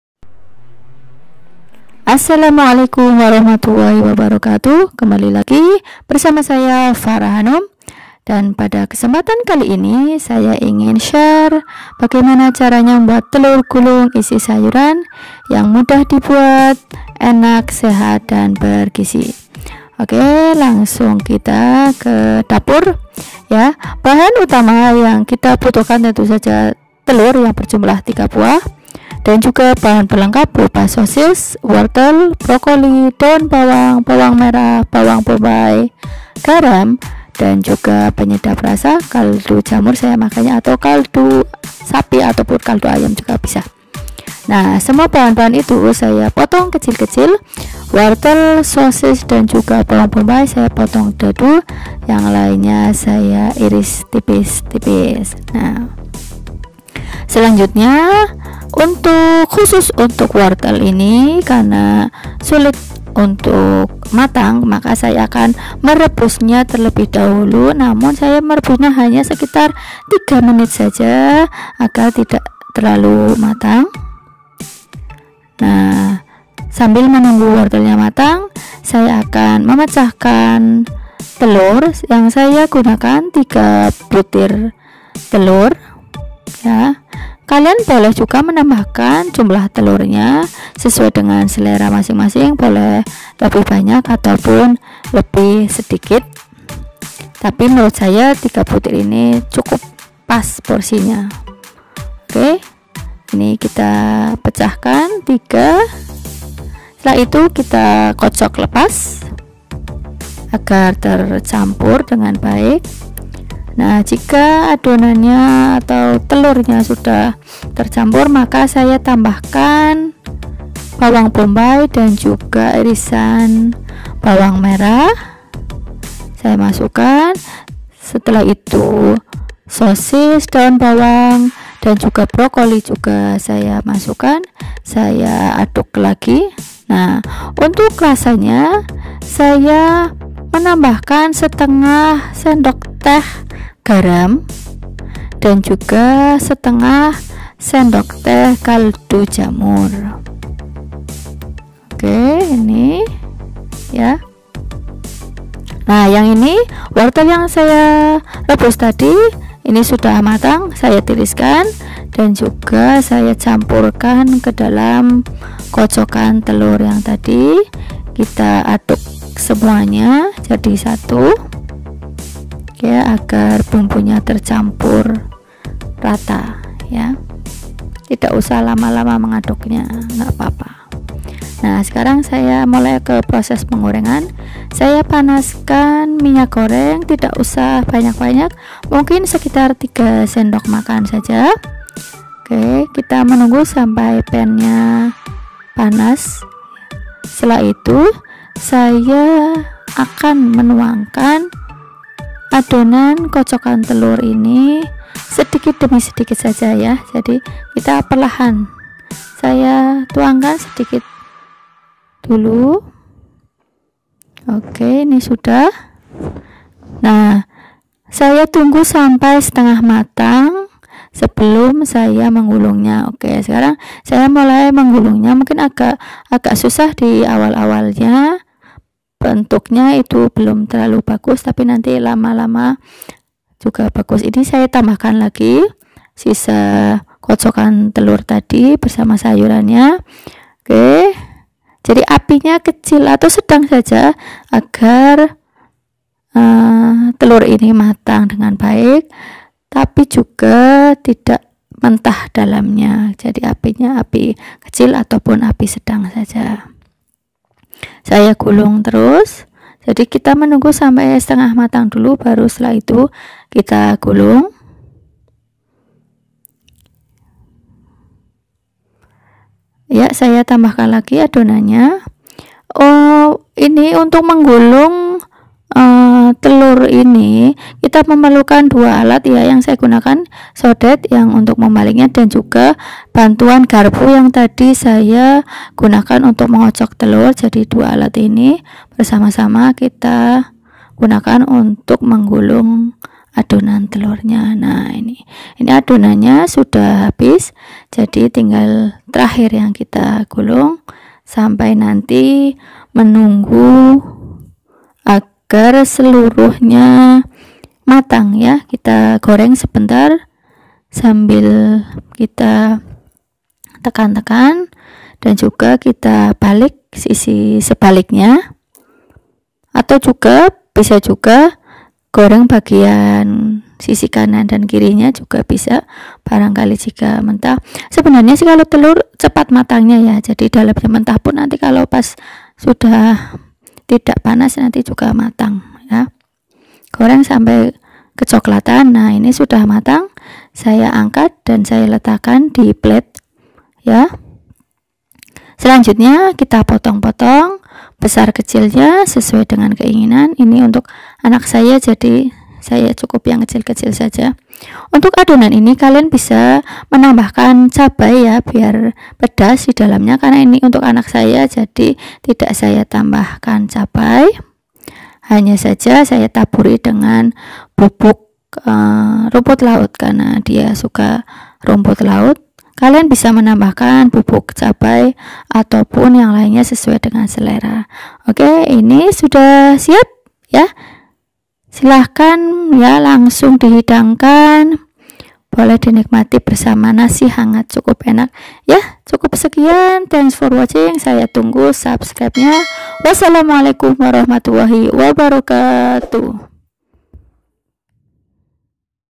Cara Membuat Telur Gulung Isi Sayuran No Ribet"...
Jika anda ingin menggoreng telur tapi ingin yang tidak biasa" aja, maka anda bisa mengcombain dengan sayuran agar lebih kaya gizi dan manfaat. Nah, bagaimakah cara membuatnya? Kalian bisa simak video berikut tentang cara membuat Telur Gulung Isi Sayuran yang nikmat, lezat, bergizi dan pastinya mudah untuk membuatnya.